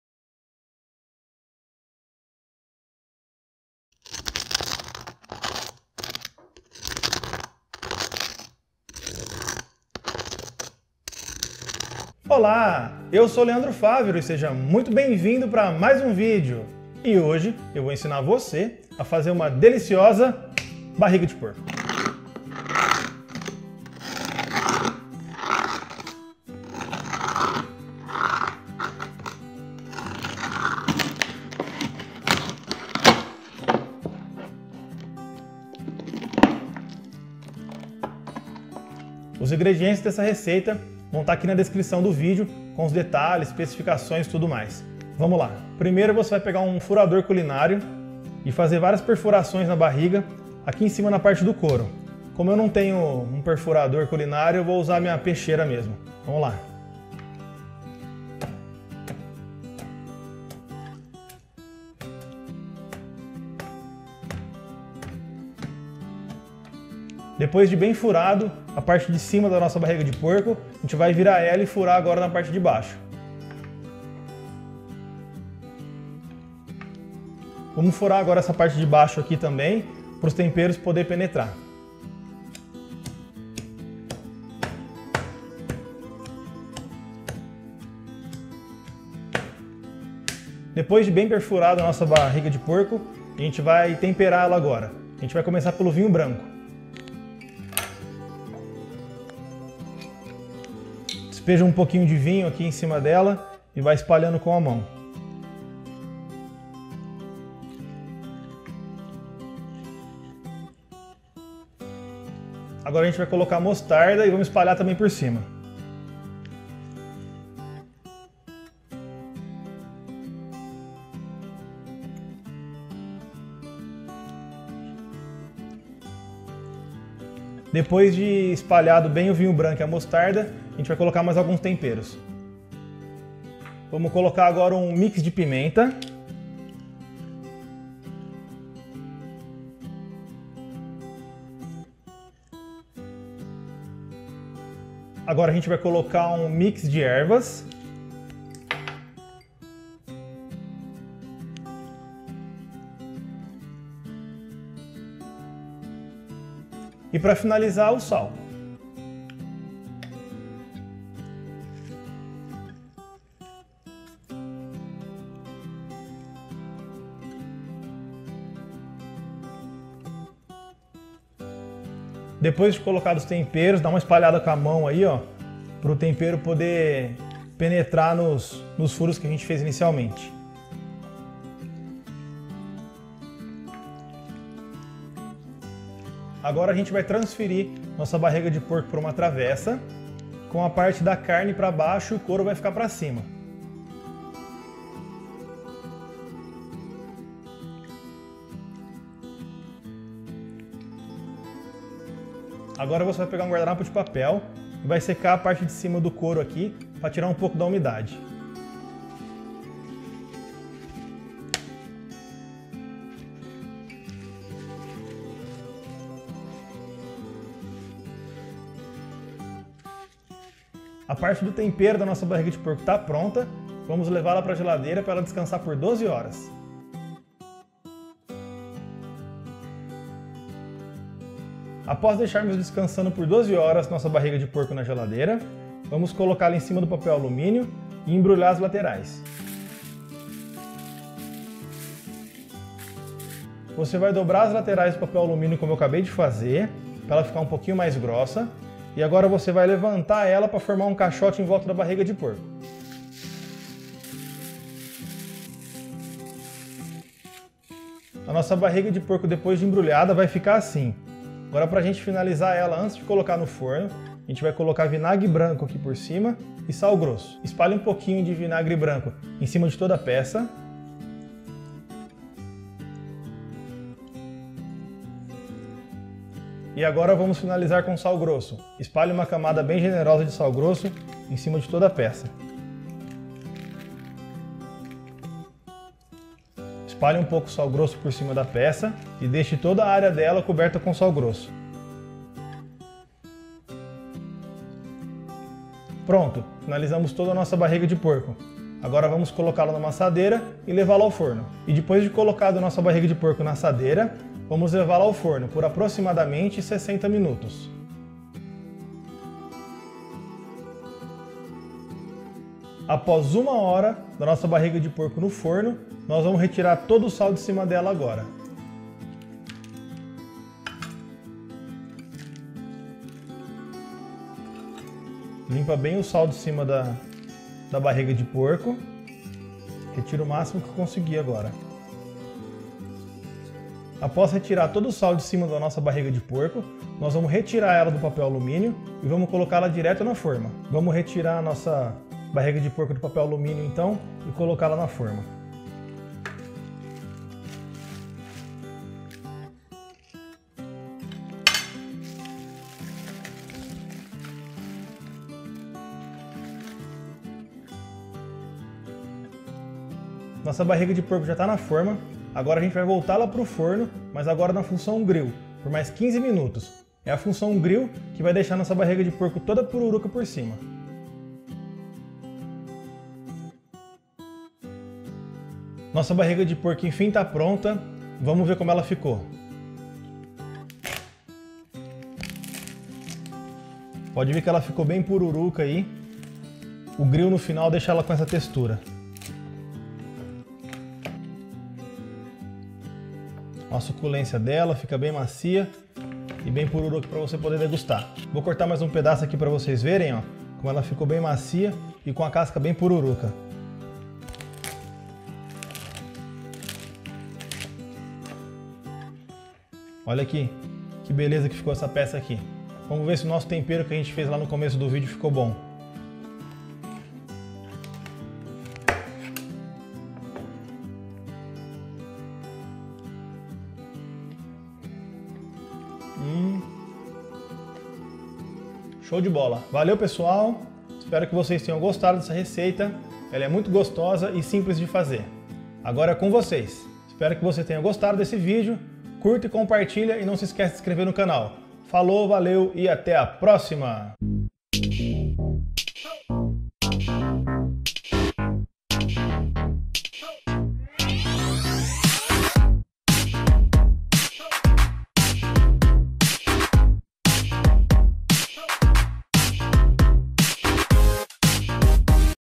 COMO FAZER BARRIGA DE PORCO
A receita desse vídeo é uma deliciosa barriga de porco!
Rápida de fazer e fica uma delícia.

Ingredientes

-Uma peça de barriga de porco.
-Vinho branco seco
-Mostarda
-Mix de pimenta
-Mix de ervas
-Sal fino
-Vinagre branco
-Sal grosso

*Temperar a seu gosto.